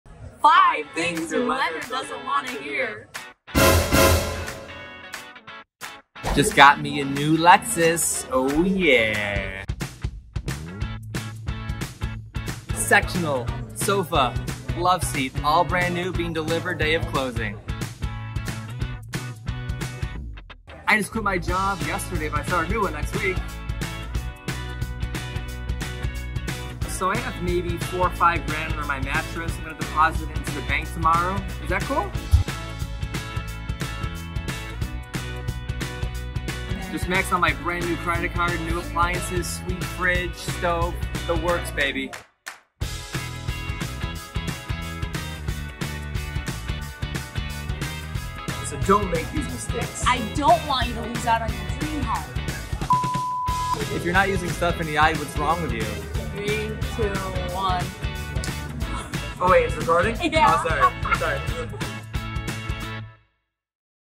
5 Things Your Lender Doesn't Want To Hear
Here Are 5 Things Your Lender Doesn't Want to Hear.
1) I just bought a new car!
2) I just bought a new furniture set and financed it all - it's going to look great in my new home!
3) I quit my job, and I'm starting my new job on Monday! Yay for new beginnings!
4) I forgot about all of that cash I had sitting around, so I deposited it into my account. That should help with closing, right?
5) The new house needed new appliances, just maxed out my card!

While some of these may sound silly... they are REAL things that people say and do before closing on a home and they can be absolute deal breakers in the home-buying process.

If you have questions about how to become home-buyer ready, give me a call. I can tell you my tips and tricks!

- Stephanie